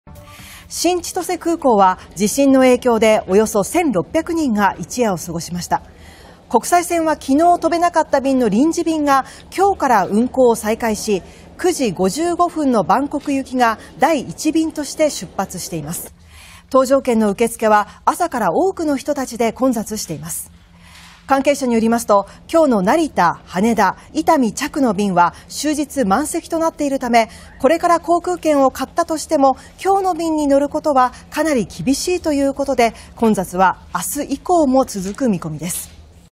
新千歳空港の国際線再開 搭乗券求め朝から混雑(18/09/08)
新千歳空港では7日の国内線に続いて8日朝から国際線の運航が再開されました。　新千歳空港は地震の影響で約1600人が一夜を過ごしました。国際線は7日に飛べなかった便の臨時便が8日から運航を再開し、午前9時55分のバンコク行きが第1便として出発しています。